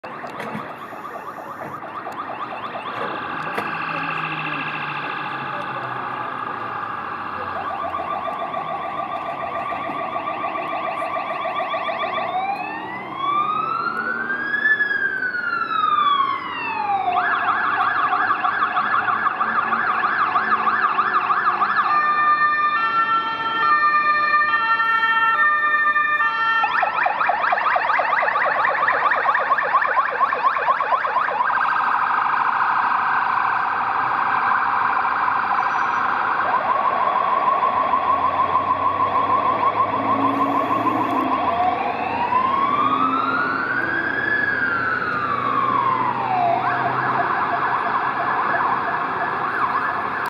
Alarmowo: Ambulans (T) - Mercedes-Benz Sprinter III/AP - PIM MSWiA Warszawa
🌍Warszawa, Rondo ONZ 📅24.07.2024 🕛00:06

🚑 Mercedes-Benz Sprinter III 🚑
- Zespół: transportowy
- Zabudowa: Ambulans Polska
- Właściciel: Państwowy Instytut Medyczny MSWiA w Warszawie
- Numer rejestracyjny: WE 9W003

🚨 Sygnalizacja świetlna 🚨
- Belka LED - Gamet ORION
- 2x lampa kierunkowa LED - Gamet SLIM LED
- Lampa LED

🔊 Sygnalizacja dźwiękowa 🔊
- Modulator: Gamet GAM 150N